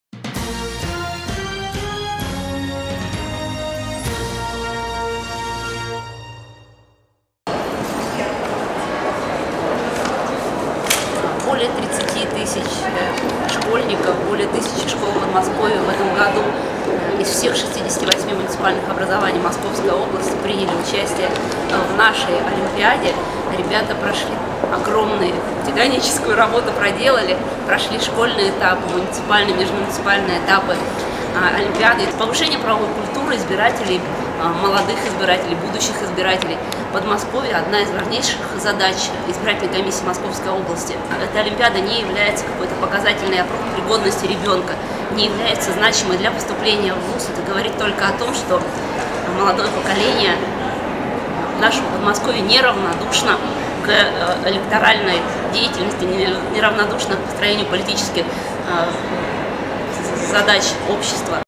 Королевские школьники признаны лучшими знатоками избирательного права
Финал ежегодной областной Олимпиады старшеклассников по избирательному законодательству состоялся вчера в Доме правительства Московской области. Победителем стала команда из городского округа Королев.